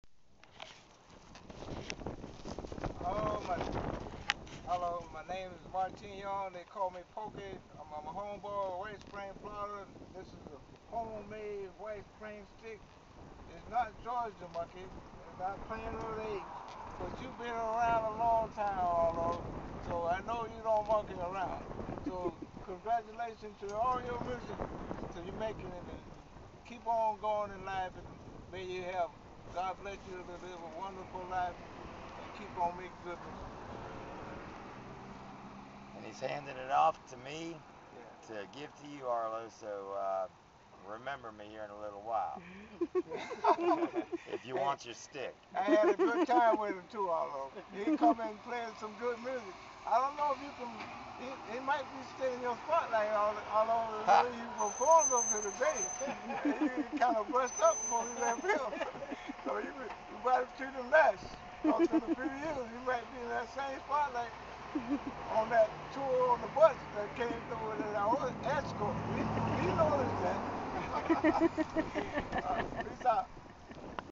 FL Folk Fest 5-26-12 (1) 006.AVI
Martine "Porky" Young -- folk artist, poet, and walking-stick maker from White Springs, Florida. This is a video "card" my wife recorded of him telling Arlo about the walking-stick he gave me to present to Arlo Guthrie at the 60th annual Florida Folk Festival, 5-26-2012.